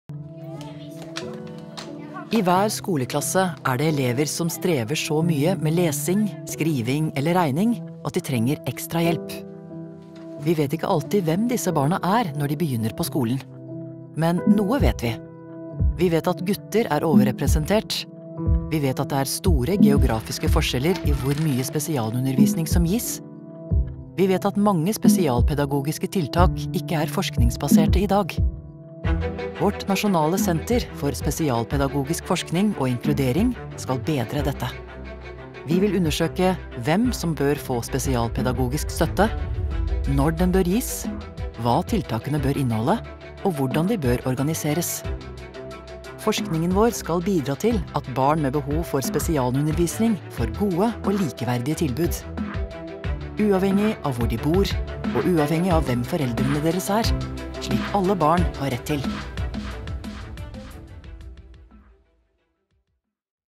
Senter for spesialpedagogisk forskning og inkludering (SpedAims)
SpedAims' ambisjon er å bedre læring, trivsel og motivasjon for barn med særskilte behov gjennom spesialpedagogisk forskning av høy kvalitet.